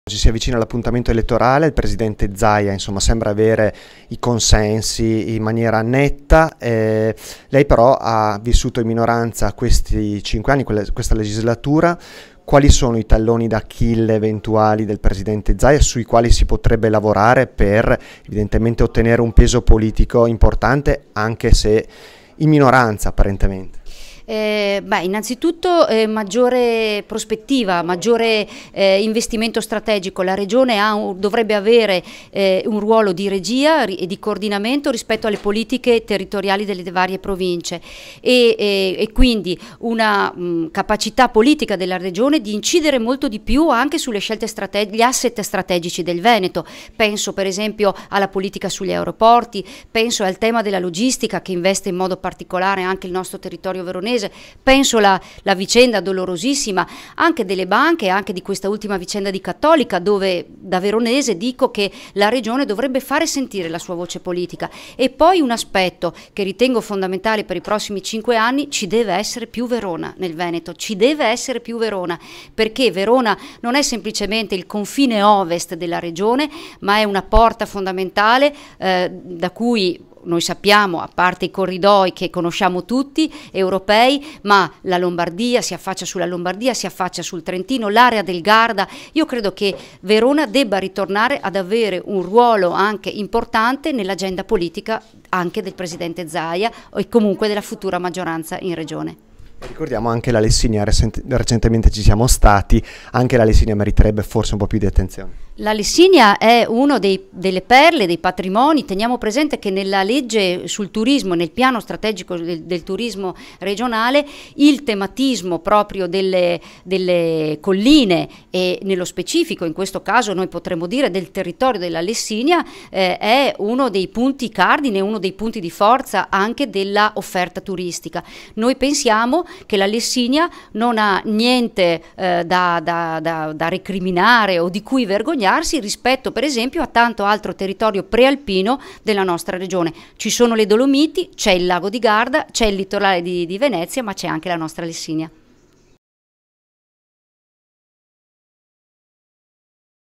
Salemi: «Nel Veneto dei prossimi anni ci deve essere più Verona»
La consigliera regionale Orietta Salemi di Italia Viva individua nella scarsa attenzione del Presidente Zaia nei confronti del territorio veronese e della Lessinia, uno dei tanti talloni d'Achille che potrebbero in qualche modo opporre resistenza a una rielezione da parte del governatore che sembra in ogni caso certa vista la sua popolarità.